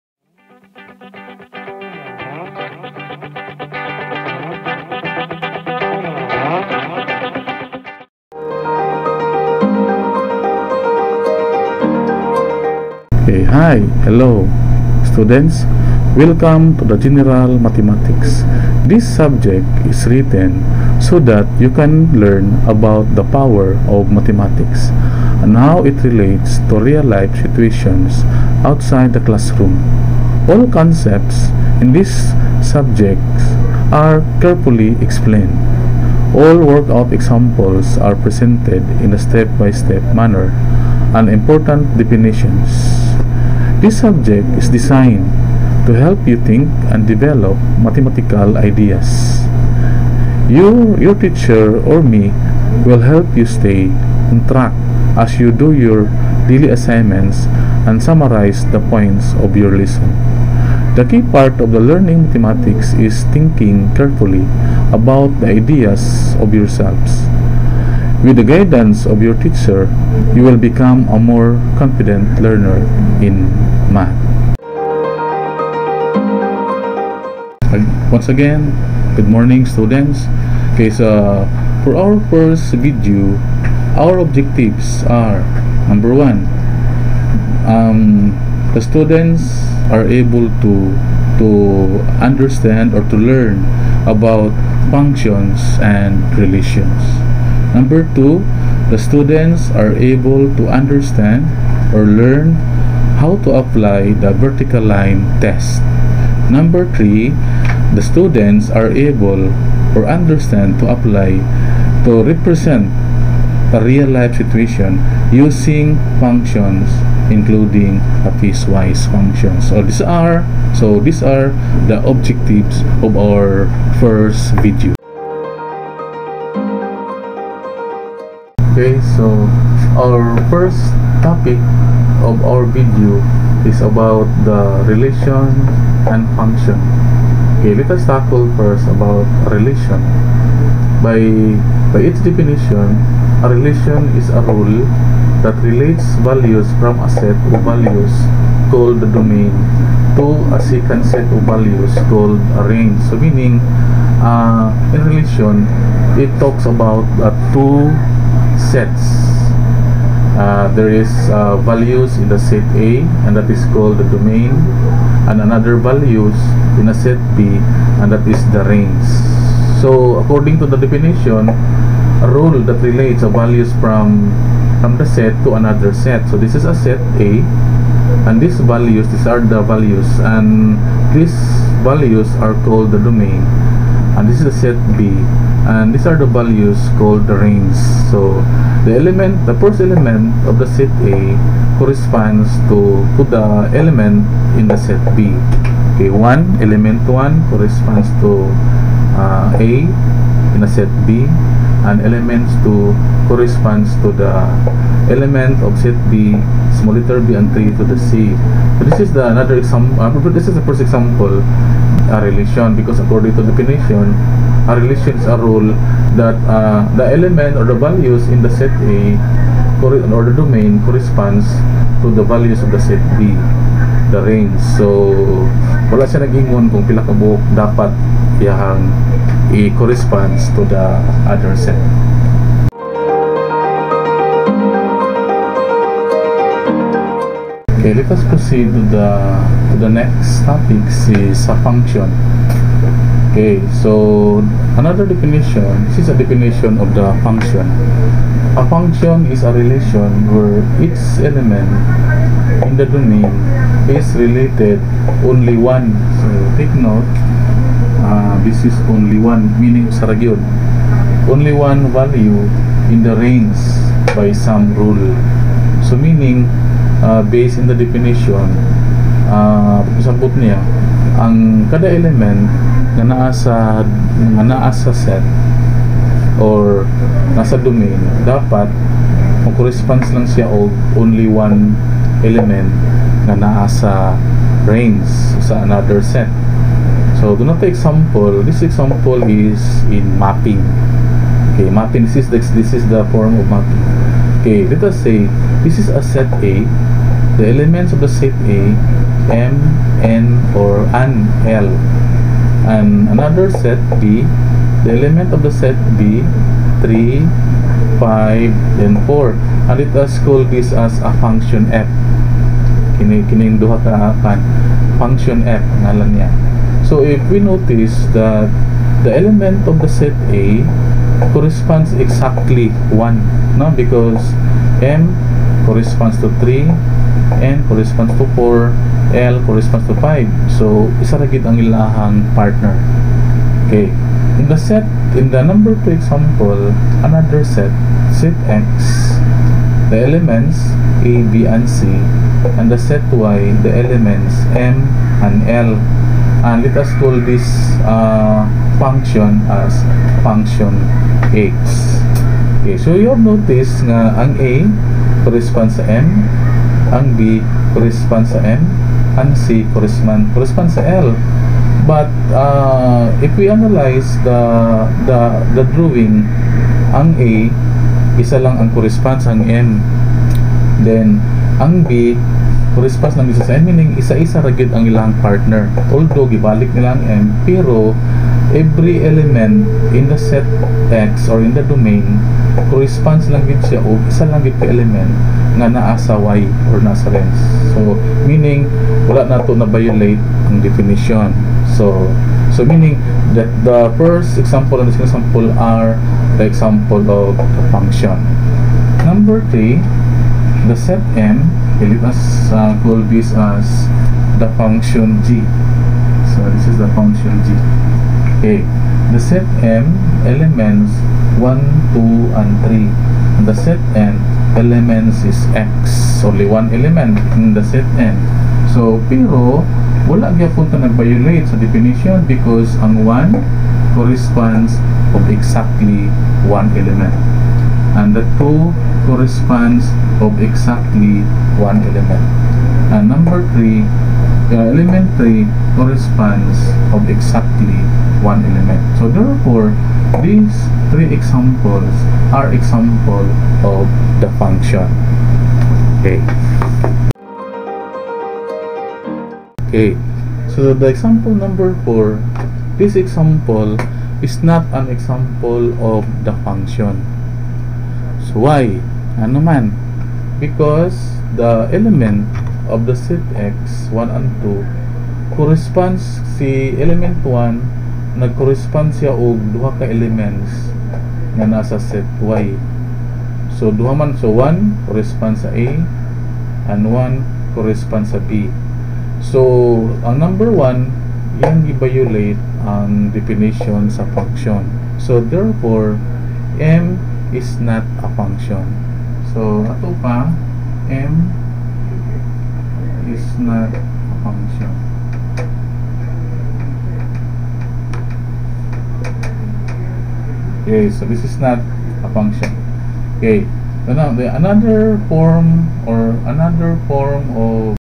Relation and Function: Domain by Mapping
Grade 11 General Mathematics
This video focuses on to identify the domain of a function through mapping.